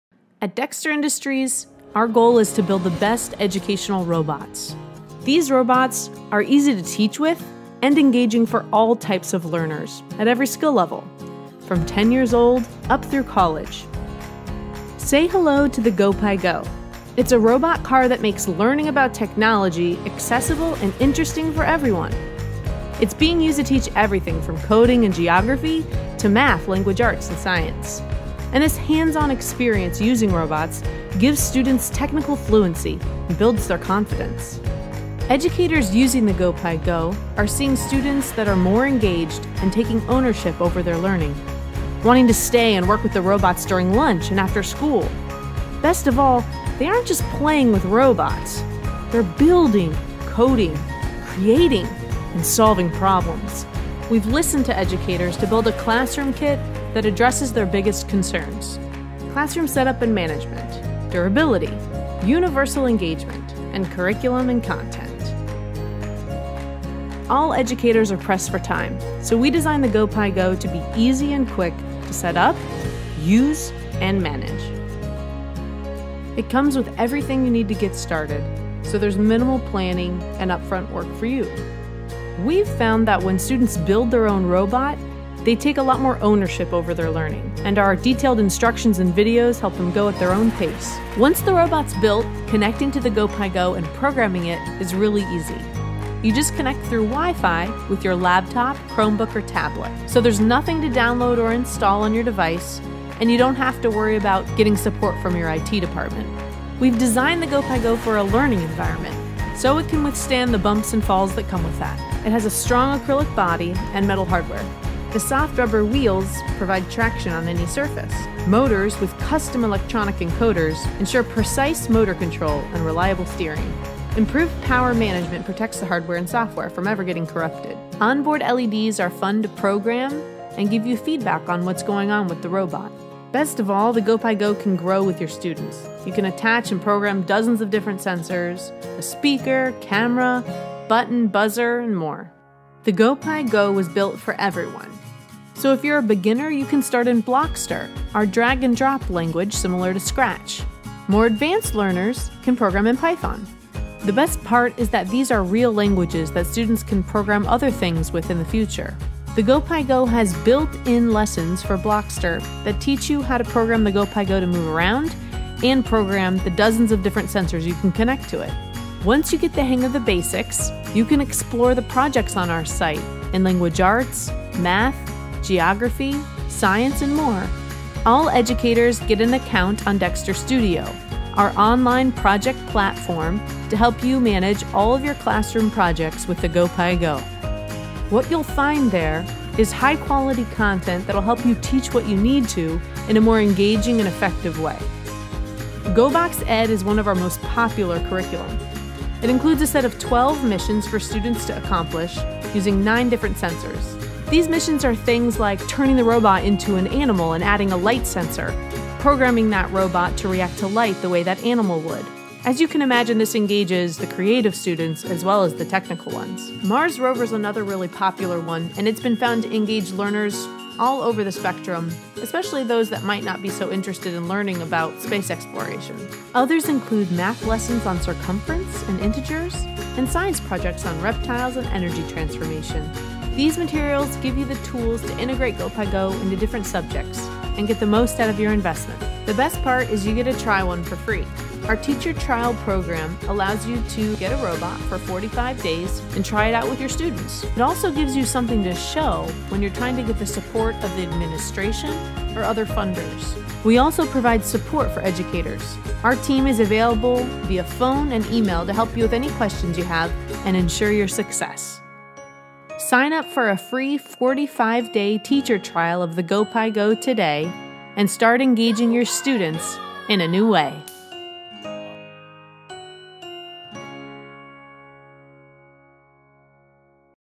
GoPiGo3 Robot for Education
Dexter Industries develops educational robots. The GoPiGo3 is a robot car designed to make learning about technology more accessible and engaging for everyone. Learn how it is being used by educators to teach everything from programming and science, to language arts and math.